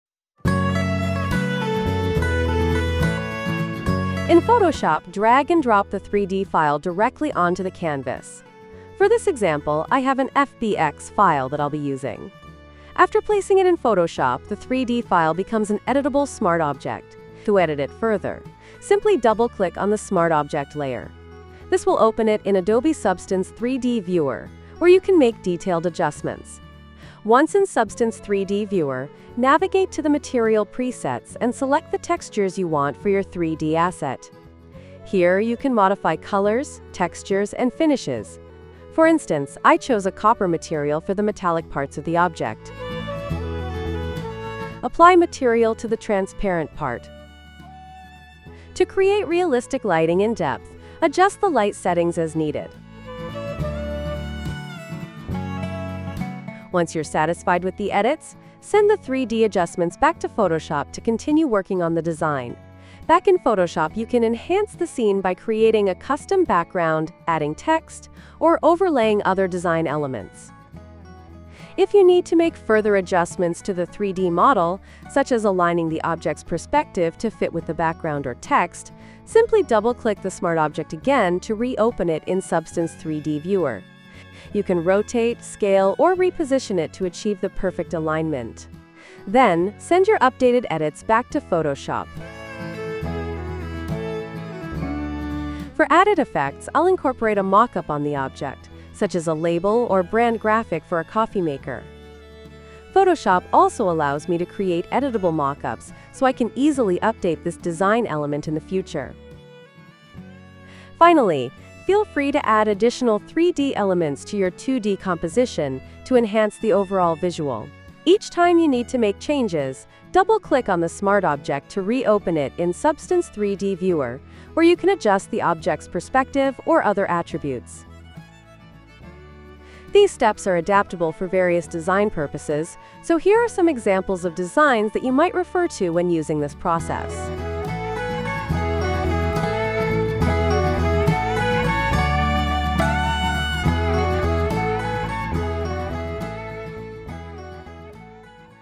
Photoshop 2025 and Substance 3D Viewer workflow
Adobe Substance 3D Viewer, a new, lightweight 3D desktop application that you can use to open, view, and display over 70 file formats. Its easy workflow is within the Photoshop 2025 application, simply drag and drop a 3D model onto your canvas and it becomes a Smart Object that you can edit.